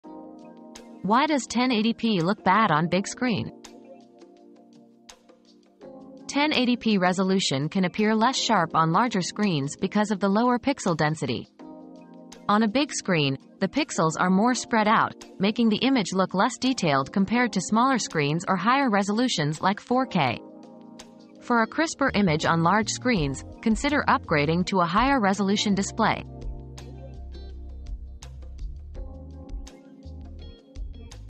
Why does 1080p look bad on big screen?
The Truth About 1080p on Big Screens • 1080p on Big Screens • Discover why 1080p resolution can disappoint on big screens and how upgrading to a higher resolution display can enhance your viewing experience.